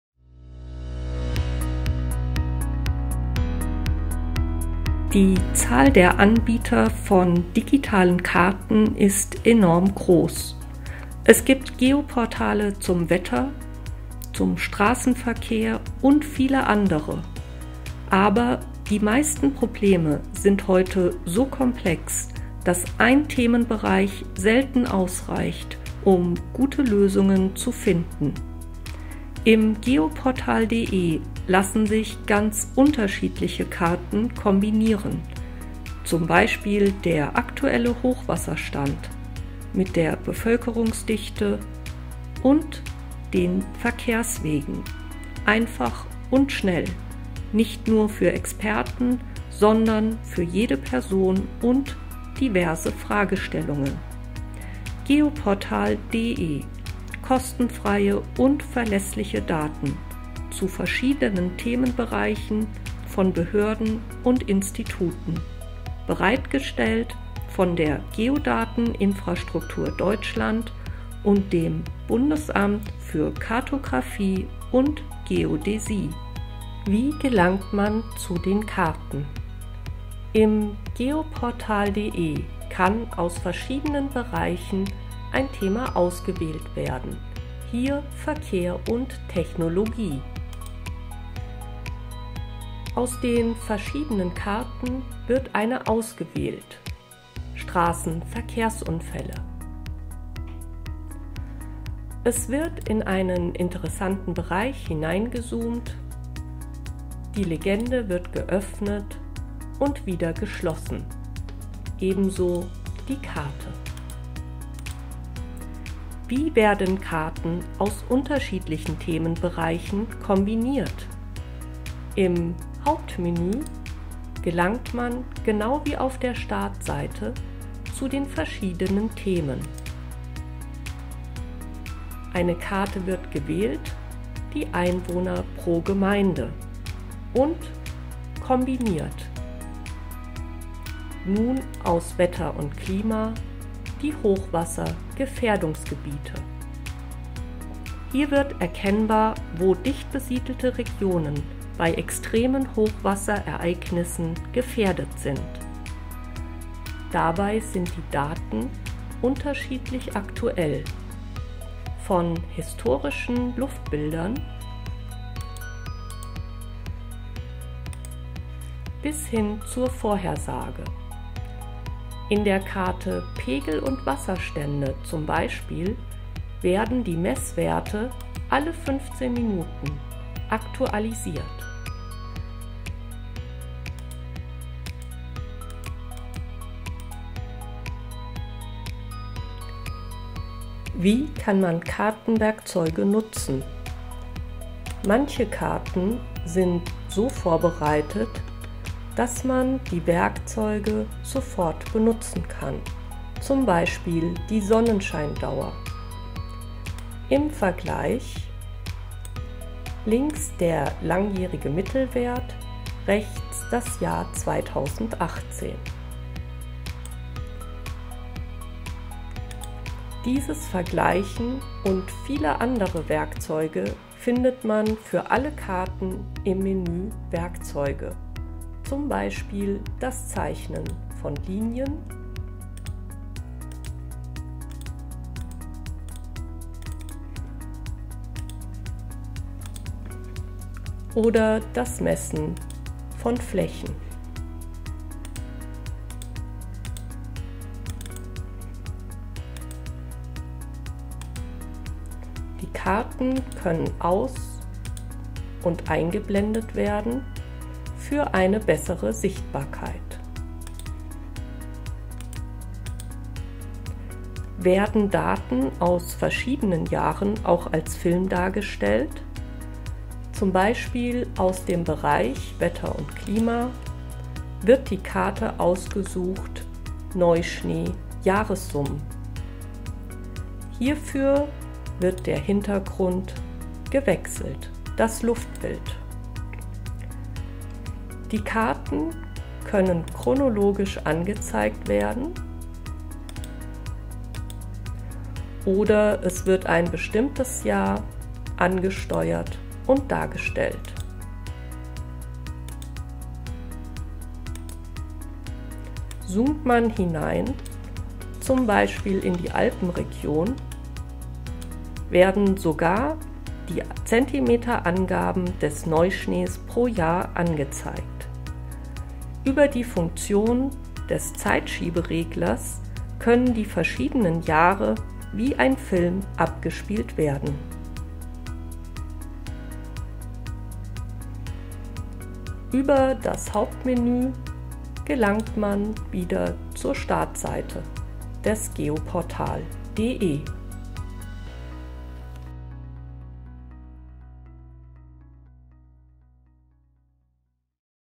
Das neue Geoportal.de
Räumliche Informationen schnell und einfach finden!

- Umfangreiches Angebot
- Einfach zugänglich
- Anschaulich präsentiert
- Zuverlässige Quellen

Sie haben Fragen, die mit „Wo“ anfangen und suchen
raumbezogene Daten (z. B. Naturschutzgebiete,
Satellitenbilder, Topographische Übersichtskarten,
Katasterdaten)?

Dann sind Sie im Geoportal.de genau richtig! Das
Geoportal.de bietet eine große Auswahl an frei zugänglichen
Geoinformationen aus den verschiedensten
Bereichen der öffentlichen Verwaltung, vom Bundesamt
bis hin zum Rathaus. Dabei reicht das Spektrum
der Daten von Wetter und Klima, Umwelt oder
Verkehr über Standort- und Navigationsdaten, die
besonders interessant für Wirtschaft und Gesellschaft
sind, bis hin zu statistischen Daten, die häufig in der
Wissenschaft und der Verwaltung genutzt werden.
In einem groß angelegten Projekt haben das Bundesamt
für Kartographie und Geodäsie (BKG) und
die Geodateninfrastruktur Deutschland (GDI-DE)
das Geoportal.de weiter für Sie ausgebaut. Ziel war
die möglichst einfache Bedienung des Portals, um die
Recherche und Nutzung der darin enthaltenen Daten
und Informationen zu erleichtern, für Bürger sowie
Entscheider aus Politik, Wissenschaft und Wirtschaft.
Hierzu wurde die Übersichtlichkeit des Portals erheblich
verbessert, der Zugang für mobile Endgeräte sichergestellt
und die Navigation intuitiv gestaltet.